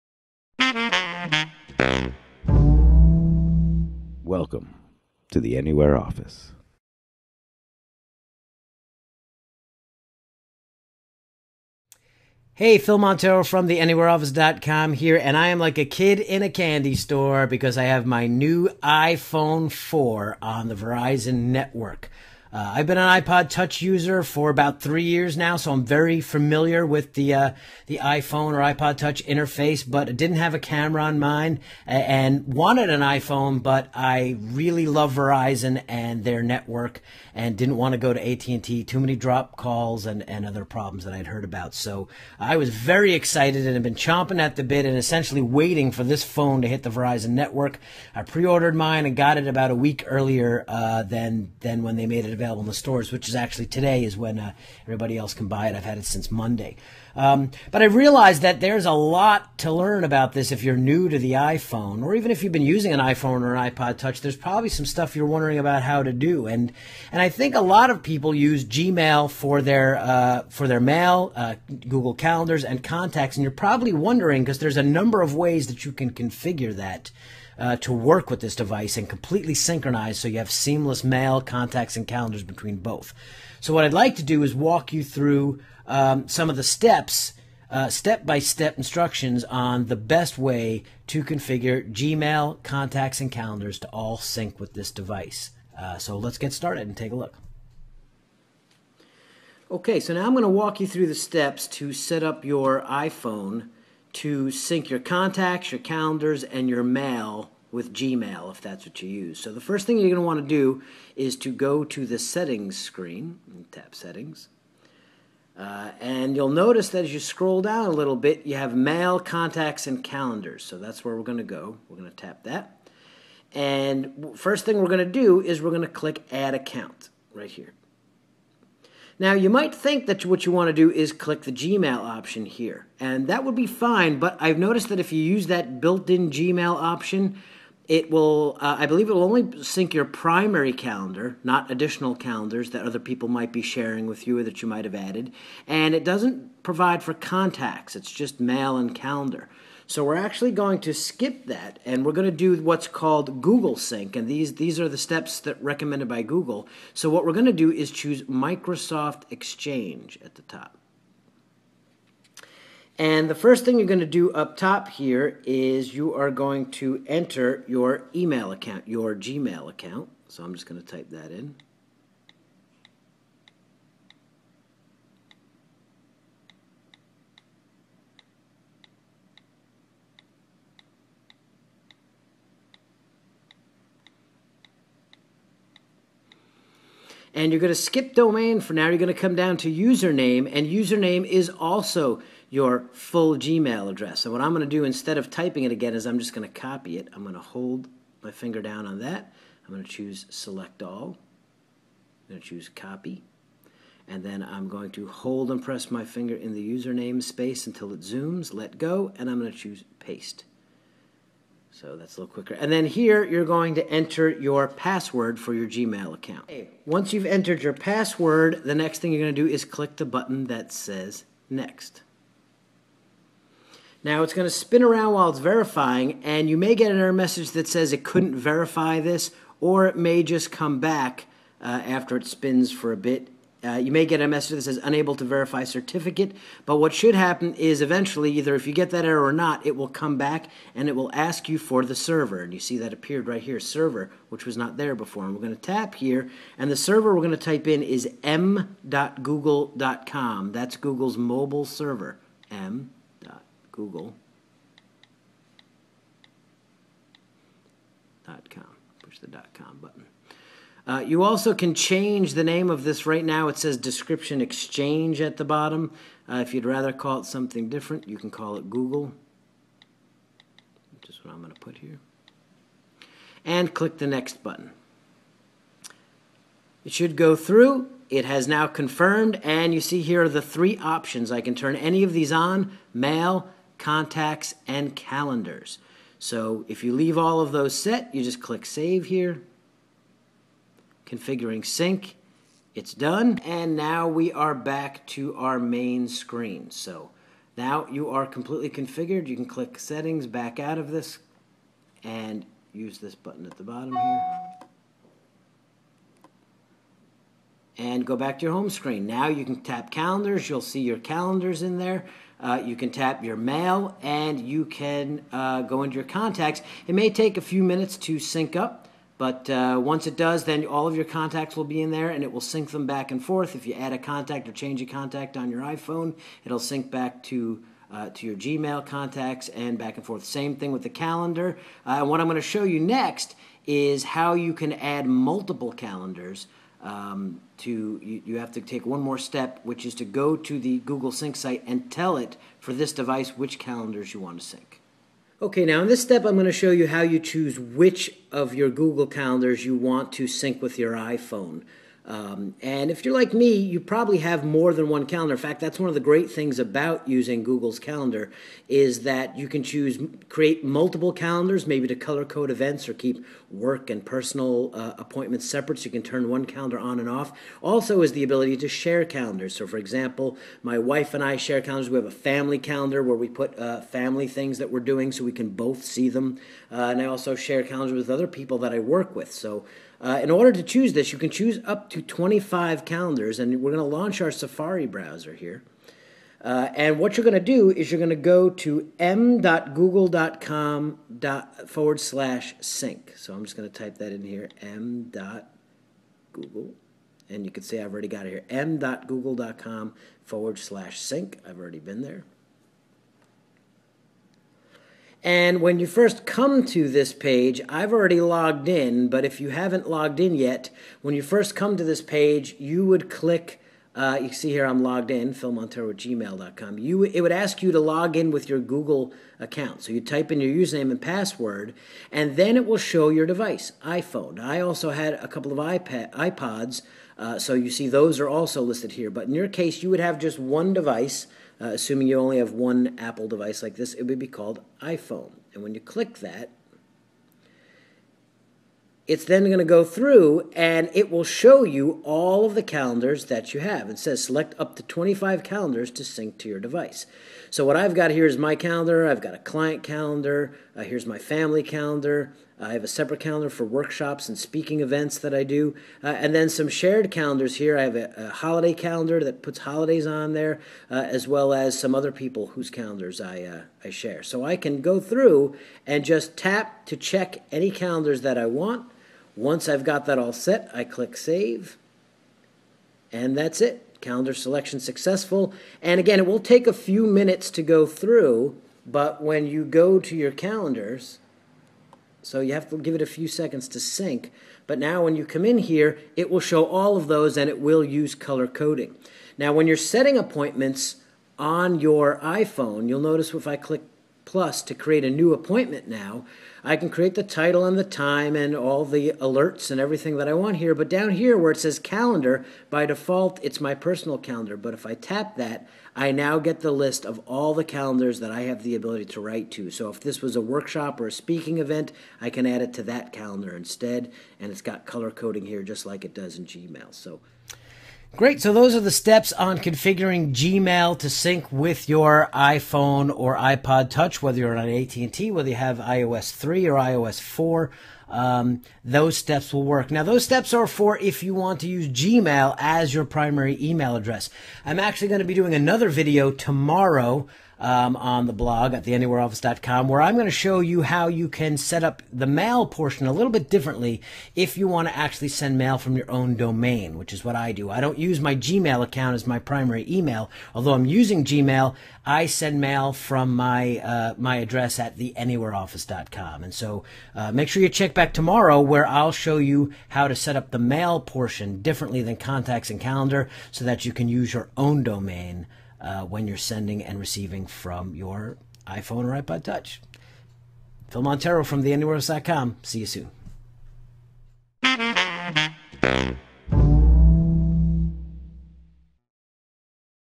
How to sync your iPhone, iPad or iPod Touch with Gmail, Google Calendar & Contacts
In this video I show you how to sync your Gmail, Google Contacts, and Multiple Google Calendars (up to 25) with your iPhone, iPad, or iPod Touch so you can send/receive mail, schedule appointments, and call any of your contacts from anywhere - instantly. Now that's The Anywhere Office in action!